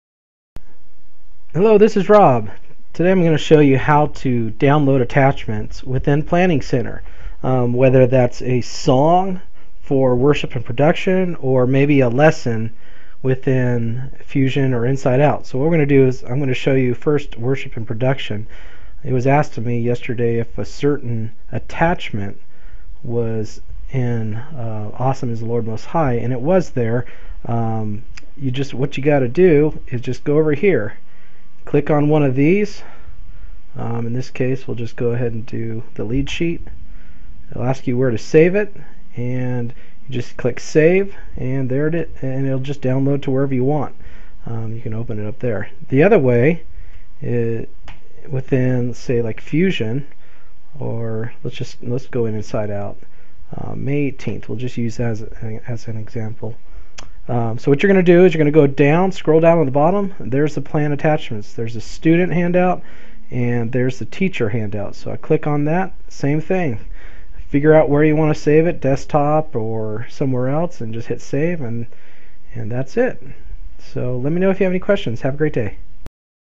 How To Download Attachments from Planning Center
How To Download Attachments from Planning Center to your computer.